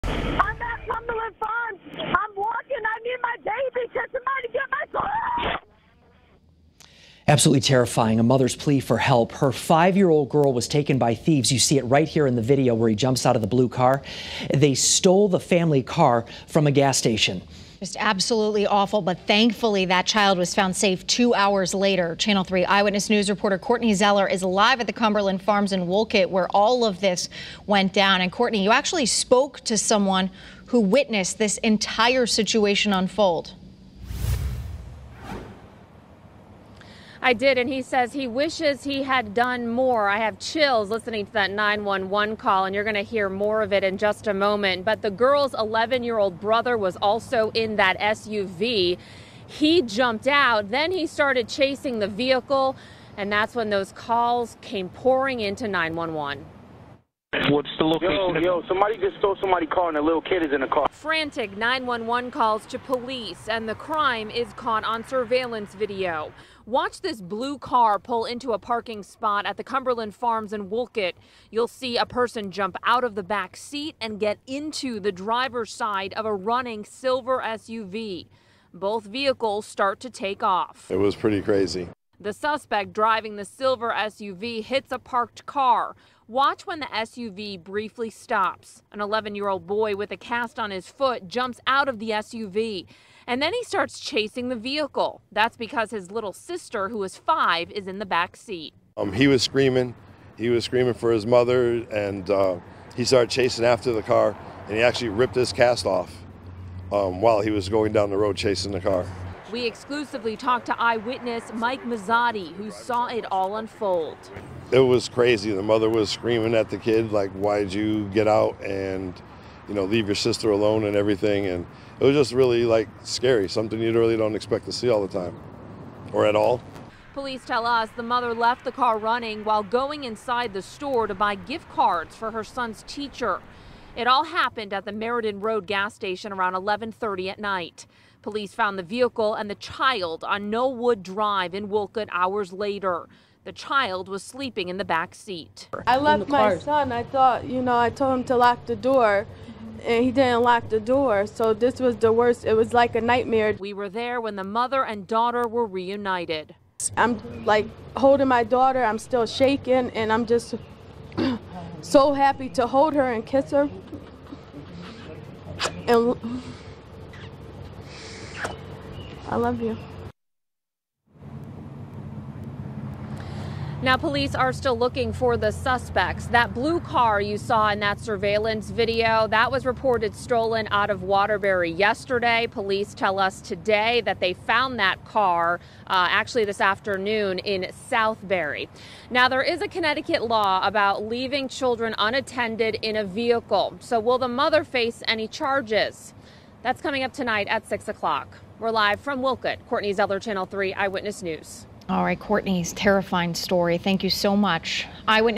VIDEO: Mother reunited with daughter after child inside stolen car in Wolcott
A 5-year-old girl was found safe early Monday morning after the car she was in was stolen by thieves in Wolcott.